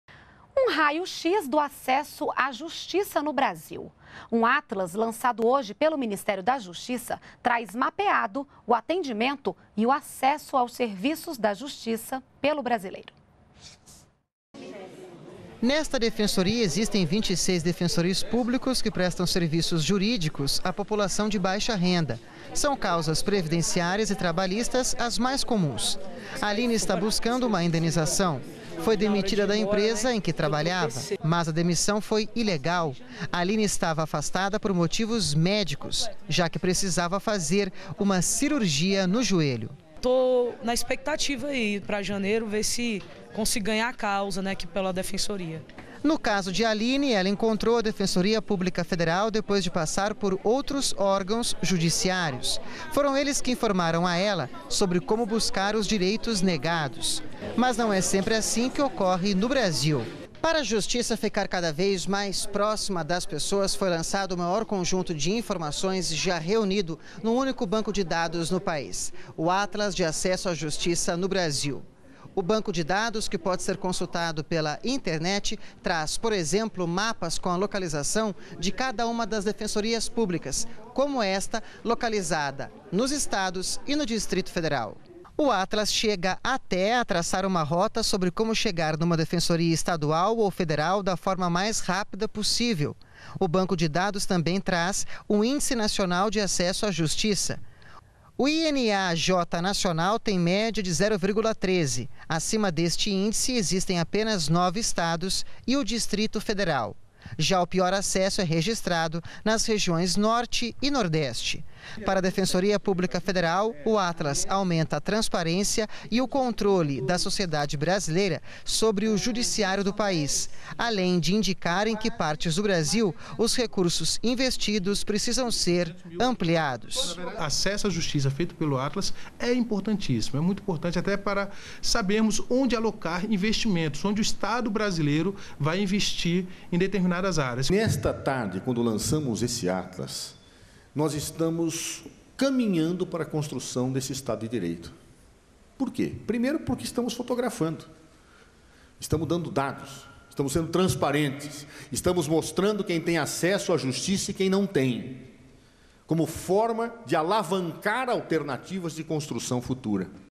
Atlas de Acesso à Justiça no Brasil vai aproximar o Poder Judiciário dos cidadãos
NBR NOTÍCIAS - 16.12.13: Nesta segunda-feira (16), o Ministério da Justiça (MJ) lançou o Atlas de Acesso à Justiça no Brasil, um banco de dados que pode ser consultado pela internet e reúne informações, por exemplo, de mapas das defensorias públicas localizadas nos estados e no Distrito Federal. O objetivo é aproximar a Justiça dos cidadãos. Para a Defensoria Pública Federal (DPF), o atlas aumenta a transparência e o controle da sociedade brasileira sobre o Poder Judiciário no Brasil. Segundo o ministro da Justiça, José Eduardo Cardozo, com o atlas, o país está caminhando para um estado de Direito, mostrando quem não tem acesso à Justiça no país.